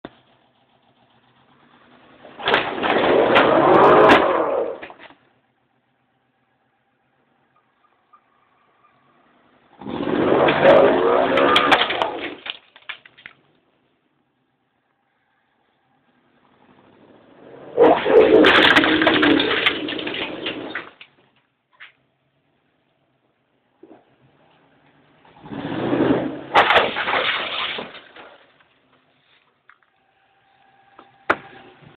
Van yanking 1
more video of me yanking that stupid van out. No front tires or struts